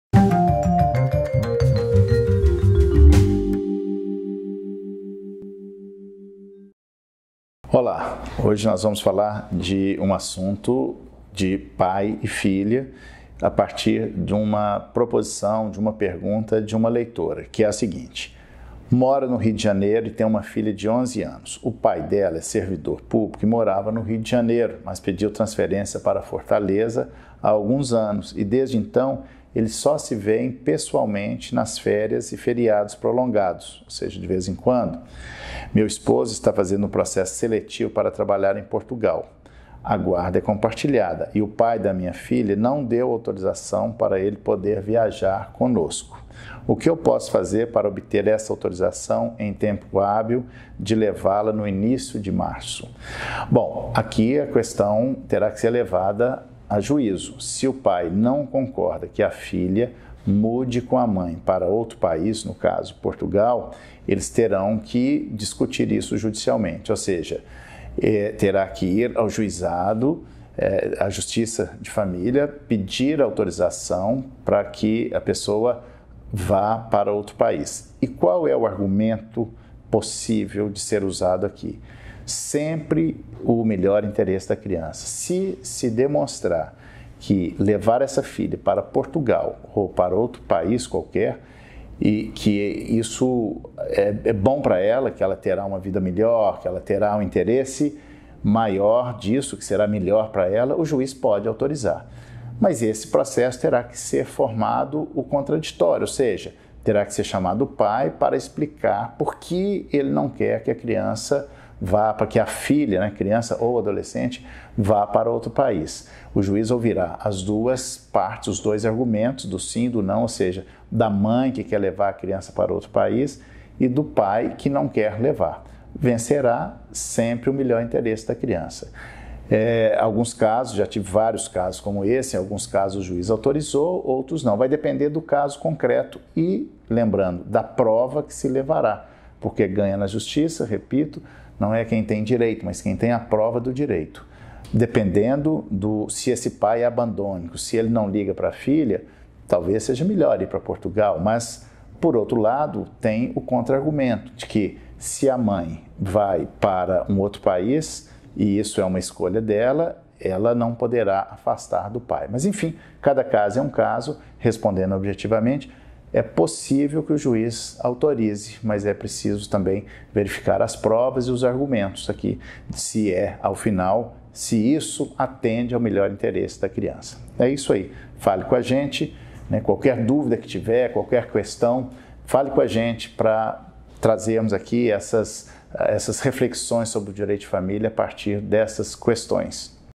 O pai da minha filha não deu autorização para poder viajar, o que fazer?
No episódio de hoje do programa Diálogos do Direito e Família o advogado Rodrigo da Cunha Pereira, especialista em Direito de Família e Sucessões, responde a uma questão de uma seguidora sobre autorização de viagem ao exterior. Confira.